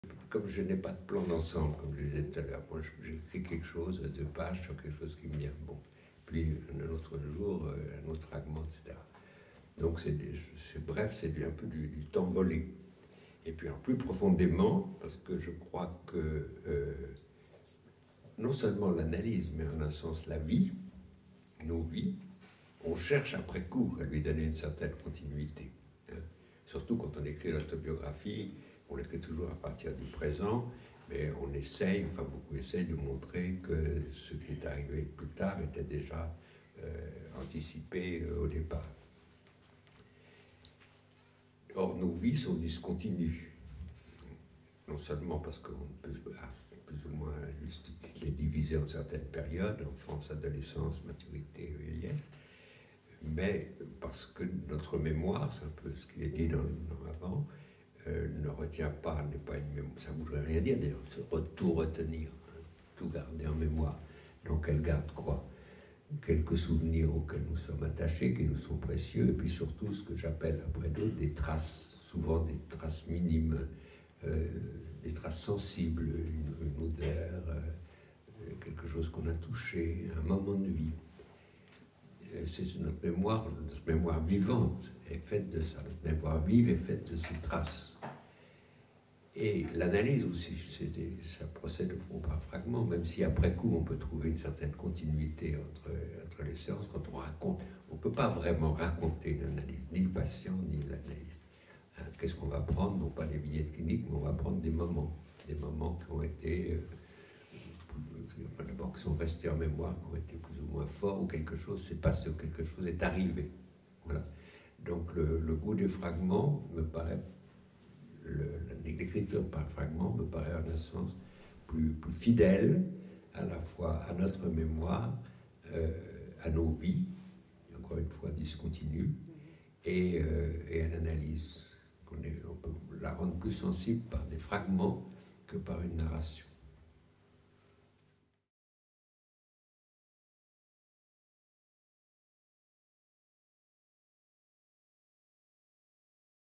Les fragments de la memoire - JB Pontalis aux vendredis littéraires des ateliers Elisabeth Bing
Réception de JB Pontalis aux vendredis littéraires des ateliers d'écriture Elisabeth Bing.
Où il est question de la fragmentation de la mémoire, de la discontinuité de la vie ...
Entretien avec Isabelle Mercat-Maheu